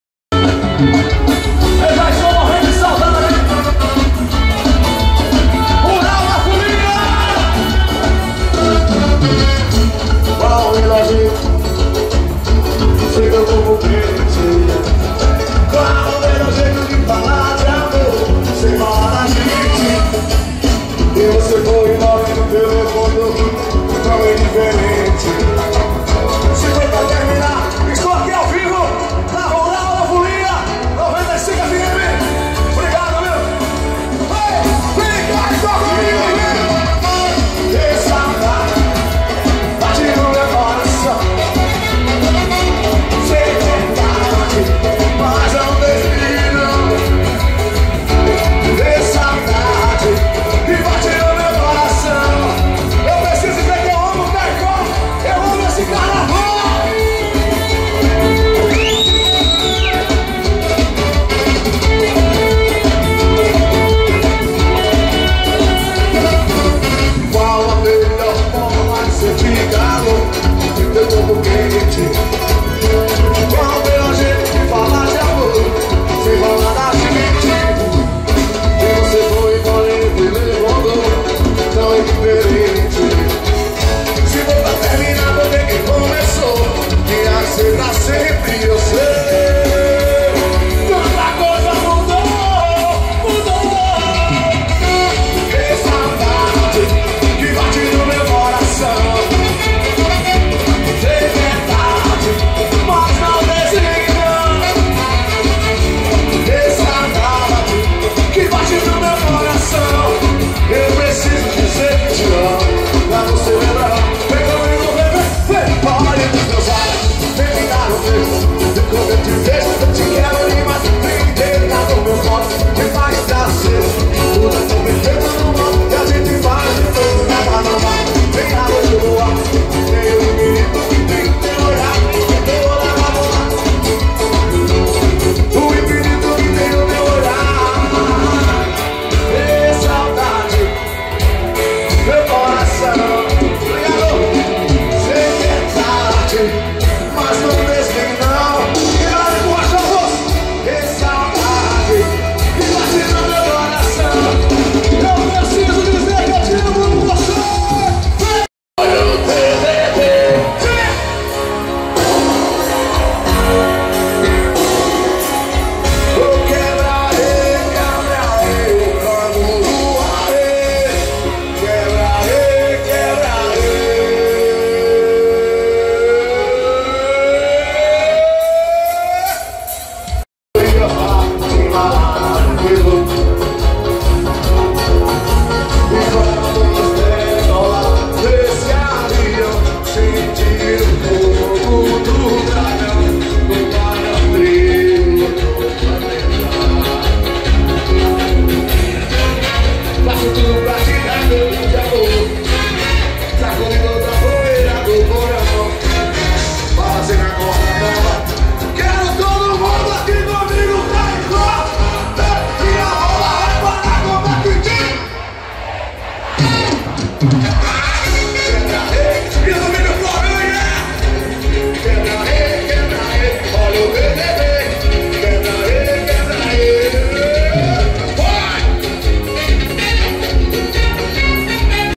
Confira como foi a paradinha do Canguru em frente ao Camarote do Sistema Rural de Comunicação.
A animação neste sábado (18) ficou por conta de Giullian Monte.